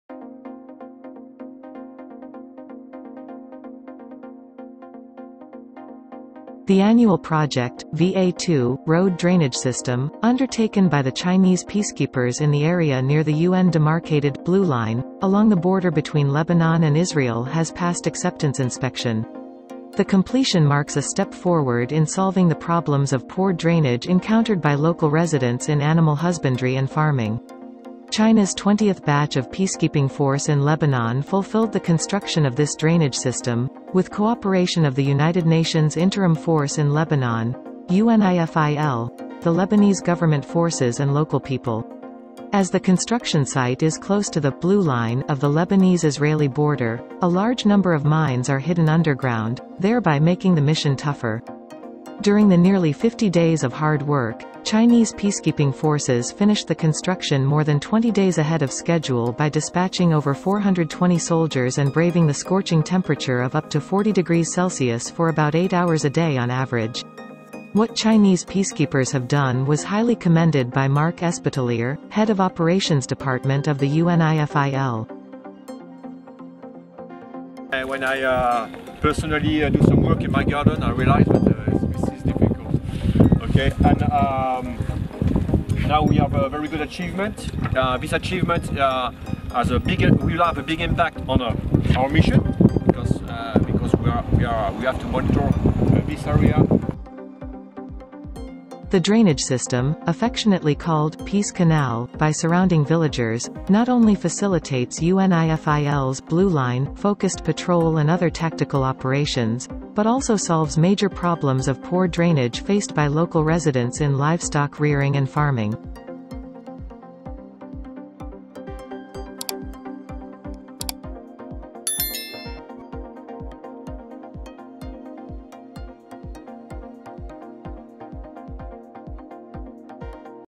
Chinese UN peacekeepers complete construction of road drainage system in Lebanon 中國聯合國維和人員在黎巴嫩完成排水系統
The annual project "VA-2" road drainage system, undertaken by the Chinese peacekeepers in the area near the UN-demarcated "Blue Line" along the border between Lebanon and Israel has passed acceptance inspection.

The completion marks a step forward in solving the problems of poor drainage encountered by local residents in animal husbandry and farming.

China's 20th batch of peacekeeping force in Lebanon fulfilled the construction of this drainage system, with cooperation of the United Nations Interim Force in Lebanon (UNIFIL), the Lebanese government forces and local people.

As the construction site is close to the "Blue Line" of the Lebanese-Israeli border, a large number of mines are hidden underground, thereby making the mission tougher.

During the nearly 50 days of hard work, Chinese peacekeeping forces finished the construction more than 20 days ahead of schedule by dispatching over 420 soldiers and braving the scorching temperature of up to 40 degrees Celsius for about eight hours a day on average.

What Chinese peacekeepers have done was highly commended by Mark Espitalier, head of Operations Department of the UNIFIL.

"When I personally do some work in my garden, I realized that this is difficult. And now, we have a very good achievement. This achievement will have a big impact on our mission, because we have to monitor this area," he said.

The drainage system, affectionately called "Peace Canal" by surrounding villagers, not only facilitates UNIFIL's "Blue Line"-focused patrol and other tactical operations, but also solves major problems of poor drainage faced by local residents in livestock rearing and farming.

中國維和人員在黎巴嫩和以色列邊界沿聯合國劃定的“藍線”附近地區承擔的年度工程“VA-2”道路排水系統工程通過驗收。

項目的建成標誌著解決當地居民畜牧業排水不暢問題又向前邁進了一步。

中國第20批駐黎維和部隊在聯合國駐黎巴嫩臨時部隊（聯黎部隊）、黎巴嫩政府軍和當地民眾的配合下，完成了這一排水系統的建設。

由於工地靠近黎以邊境“藍線”，地下埋藏大量地雷，任務難度加大。

在近50天的艱苦奮戰中，中國維和部隊出動420餘人，平均每天冒著高達40攝氏度的高溫，平均每天8小時左右，提前20多天完成建設。

聯黎部隊行動部部長馬克·埃斯皮塔利埃對中國維和人員的所作所為給予了高度評價。

“當我親自在我的花園裡做一些工作時，我意識到這很困難。現在，我們取得了非常好的成績。
這一成就將對我們的使命產生重大影響，因為我們必須監控這一領域，”他說。

這條被周邊村民親切稱為“和平運河”的排水系統，不僅方便了聯黎部隊以“藍線”為重點的巡邏等戰術行動，也解決了當地居民畜牧養殖面臨的排水不暢的重大問題。